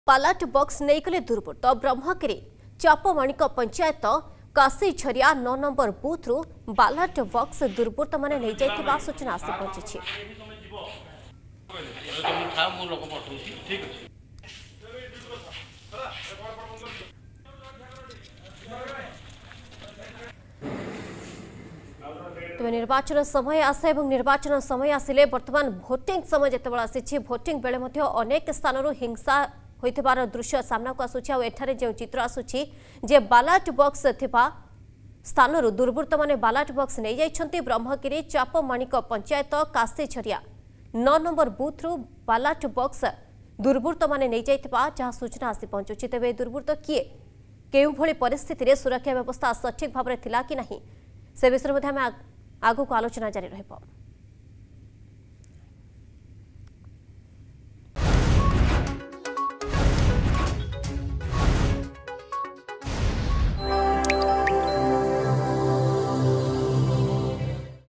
Odisha Panchayat Elections 2022: Miscreants Loot Ballot Box from Booth in Brahmagiri, Puri
Miscreants Loot Ballot Box from Booth during Panchayat Elections 2022 in Brahmagiri, Puri.